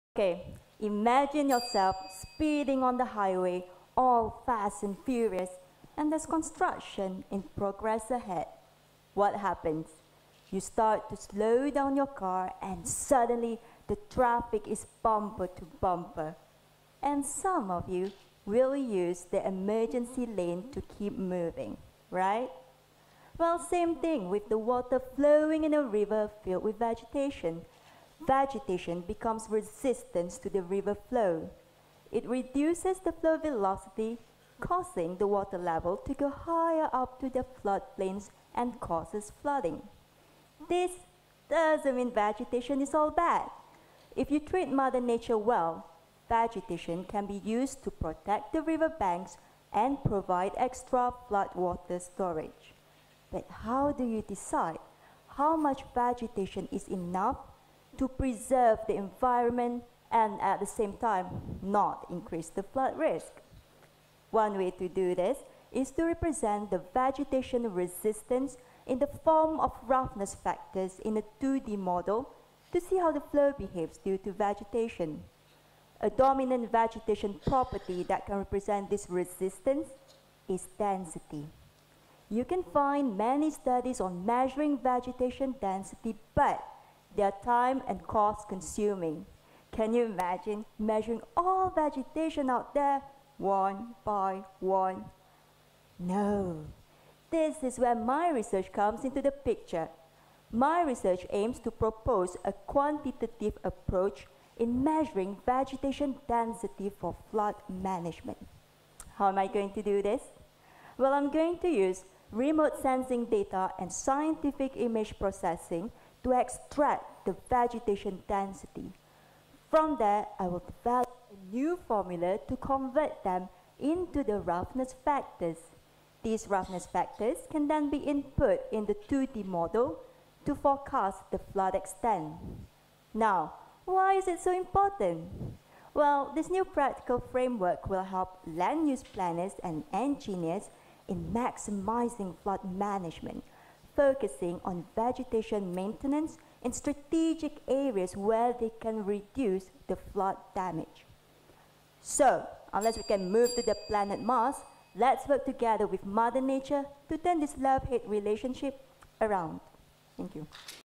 Malaysia 3MT Izni Zahidi
Izni Zahidi from Universiti Putra Malaysia has participated in the category of Engineering at the Malaysia 3MT Competition 2014 with her presentation - "Management of Vegetation & Floods: A Love-Hate Relationship" on 27 Mei 2014 in Menara Razak, UTM KL.

The Malaysia 3MT Competition aims at assessing the students' capacity to communicate ideas effectively. It is intended to enhance the development of academic and research communication skills. Selected PhD Research Students have three minutes to present a compelling oration on their thesis topic and its significance, in language appropriate to non-specialist audience.